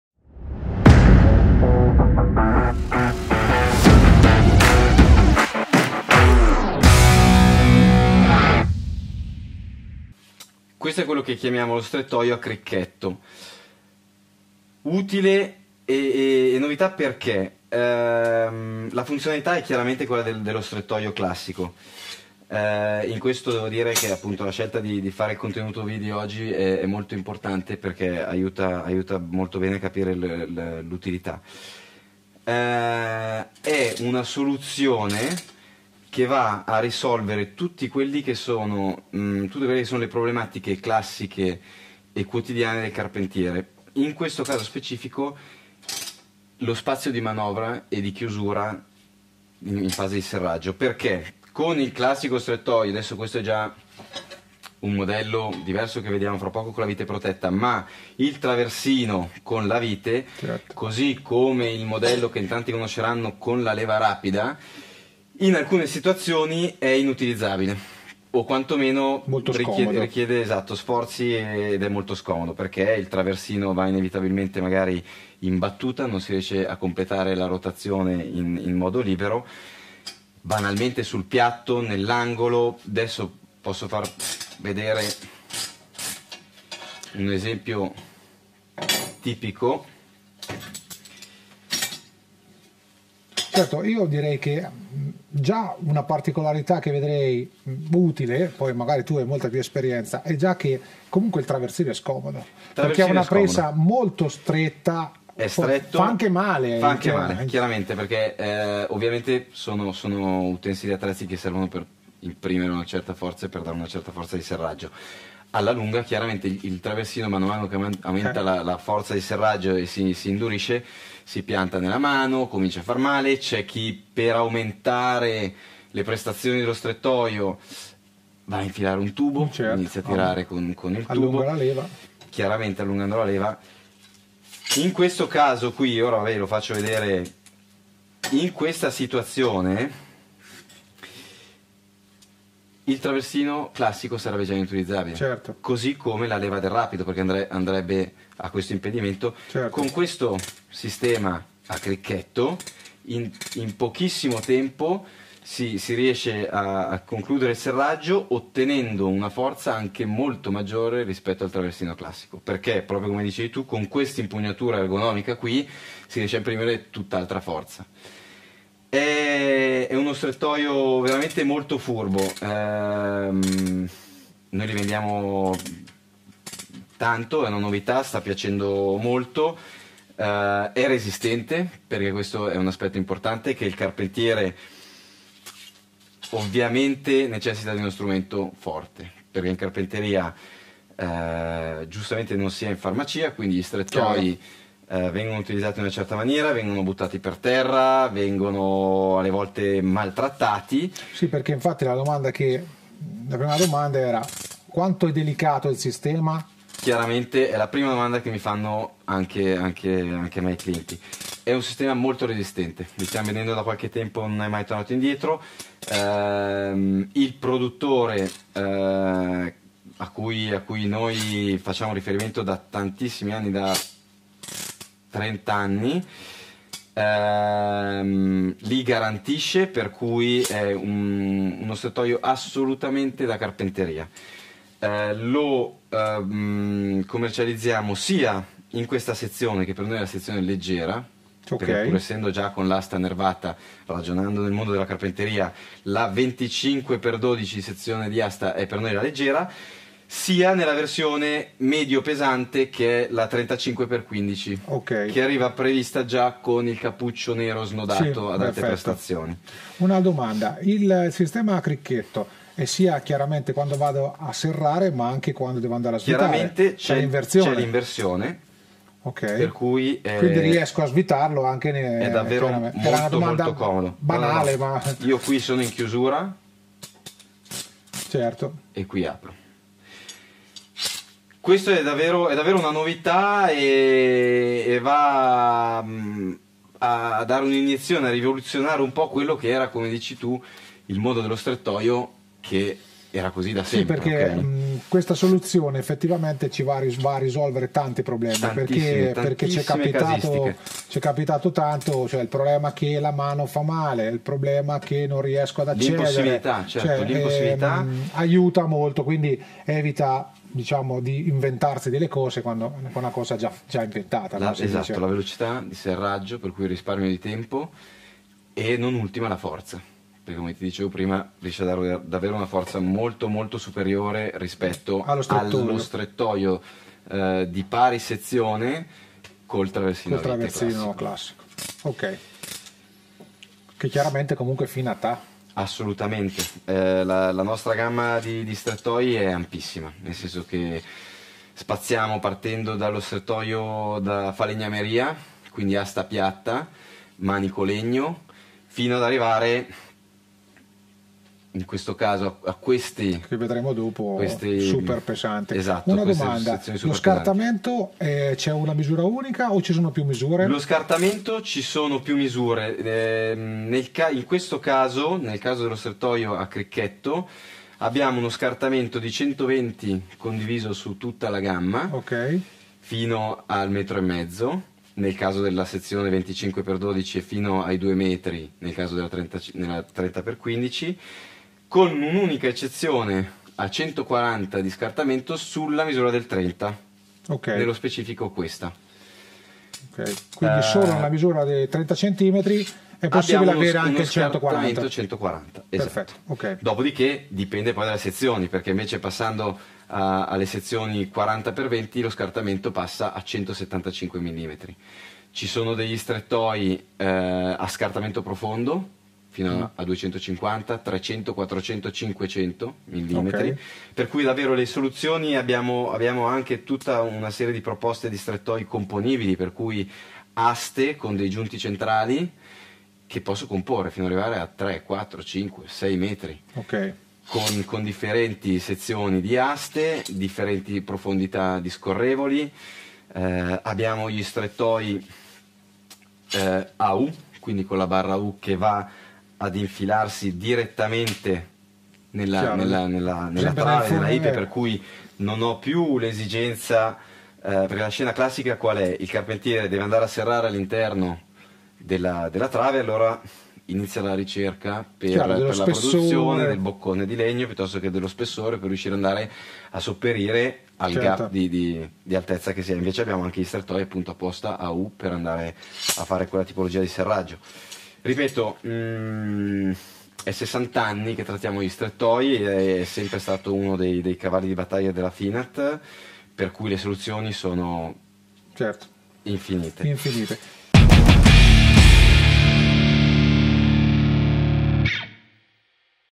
Strettoi per Carpenteria a Cricchetto
Ne abbiamo parlato con Callegari che ci ha spiegato le Particolarità di questi Strettoi ideali per le situazioni dove è difficile lavorare con gli standard a Braccetto.

Tutti i prezzi e caratteristiche  a questo link Qui Sotto.👇🏻